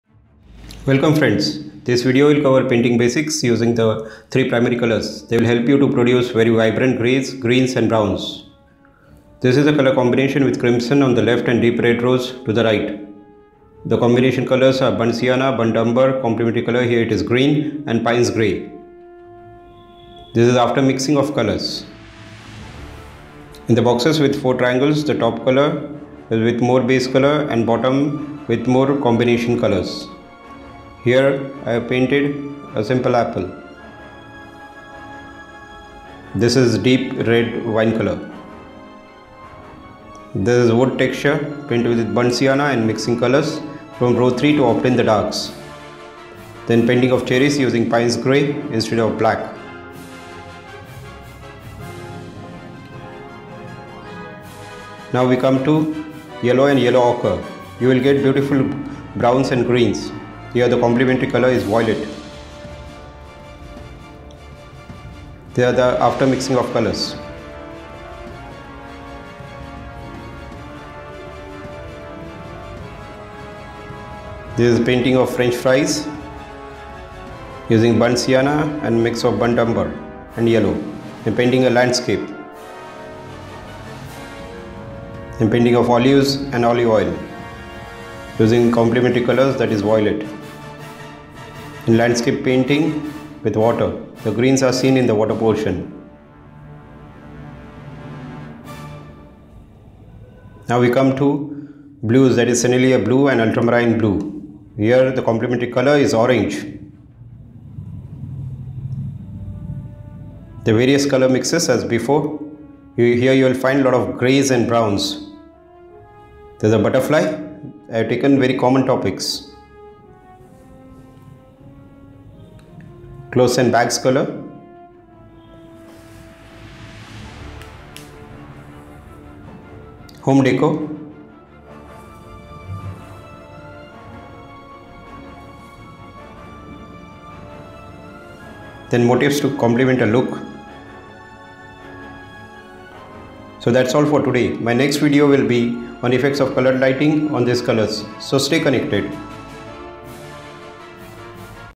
HOW TO PAINT STEP BY STEP USING THREE PRIMARY COLORS
THIS VIDEO IS A STEP BY STEP GUIDE ON WATERCOLOR PAINTING WITH PRIMARY COLORS AND THEIR MIXES WITH BURNT SIENNA, BURNT UMBER, COMPLEMENTARY COLOR AND PAYNE'S GREY. I HAVE  USED TWO DIFFERENT REDS, YELLOWS AND BLUES TO UNDERSTAND THE EFFECTS OF COLOR MIXING BETTER. THE BASIC AIM IS TO AVOID USING BLACK DIRECTLY TO MAINTAIN VIBRANCY IN THE COLOR COMBINATIONS. IT WILL HELP TO PRODUCE DIFFERENT TYPES OF GRAYS, GREENS AND BROWNS. YOU DON'T NEED TO BUY ALL SHADES OF A PARTICULAR COLOR IF YOU PRACTICE MIXING THESE COMBINATIONS.